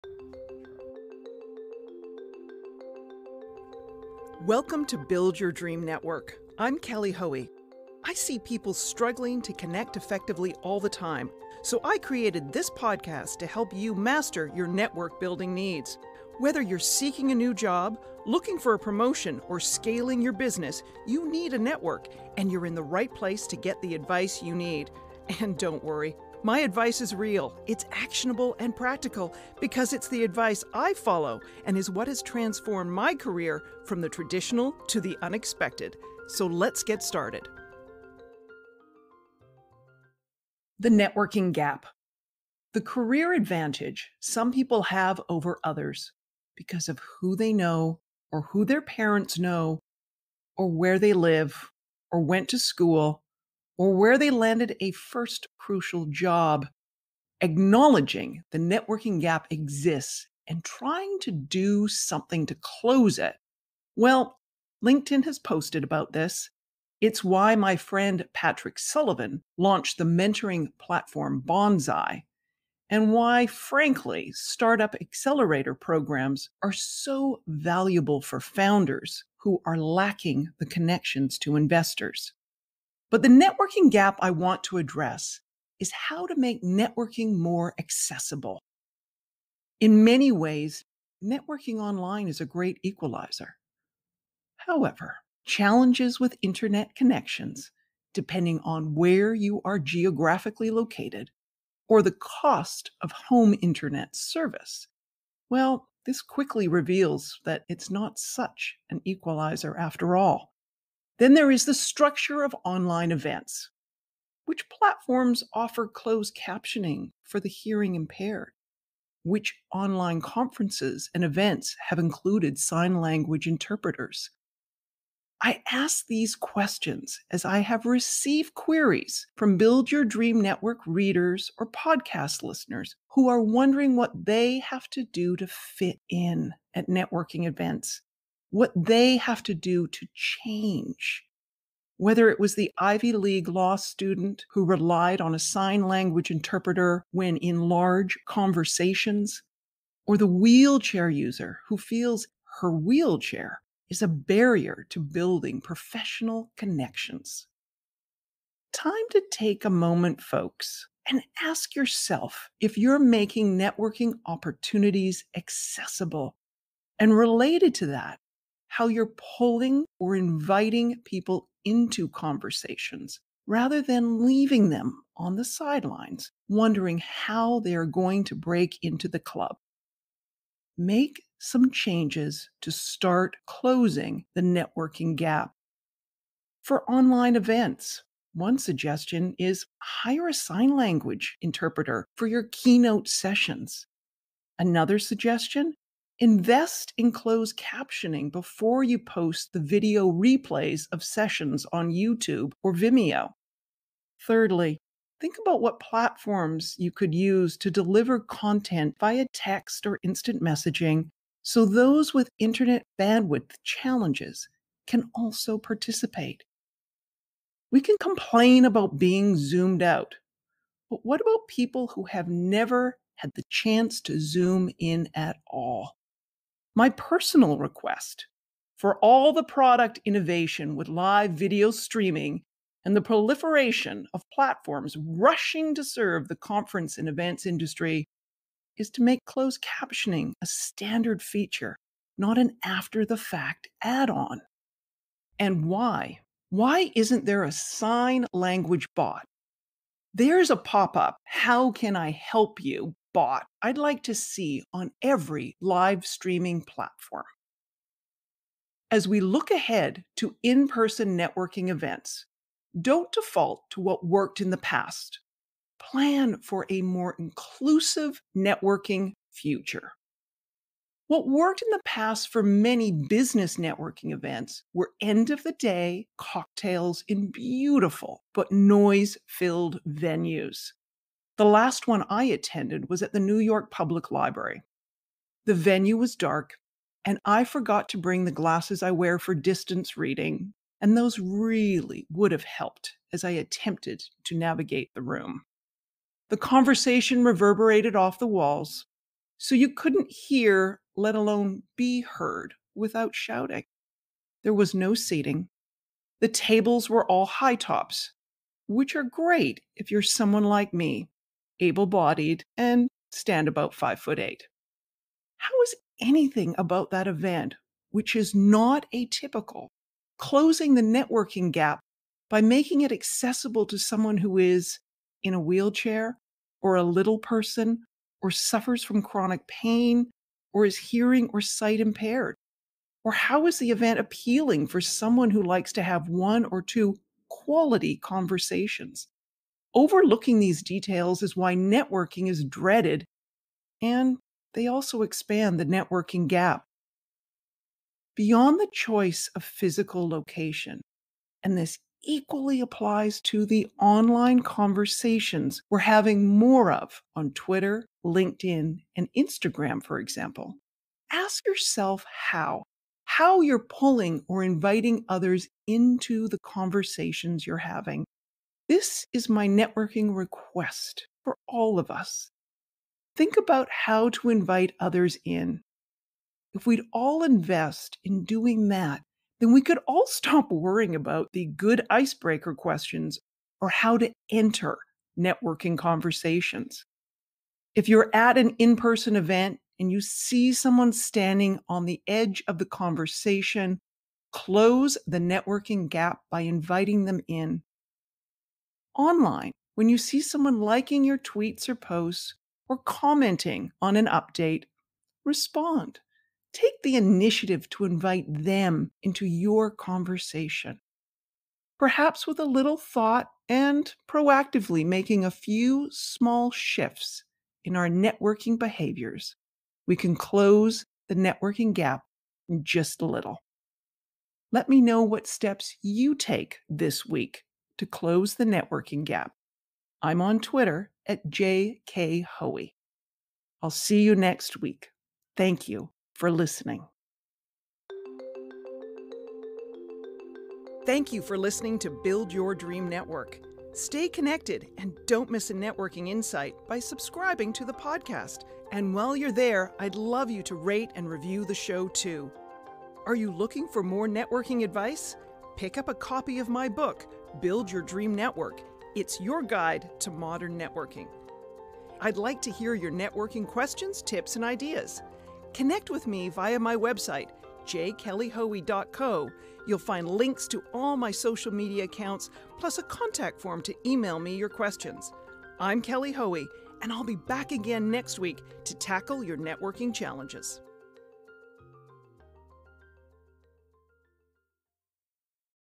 Networking Expert J. Kelly Hoey on Closing The Networking Gap
The networking gap--the advantage some people have over others based on who they know or the resources they have access to--persists even in our hyper-connected digital networking world. In this episode, Kelly addresses this gap and examines how we can start to close it, from improving the accessibility of online and in-person events to focusing more on being more inclusive and actively pulling people into networking conversations, instead of them having to muster up the courage to break the ice and join in.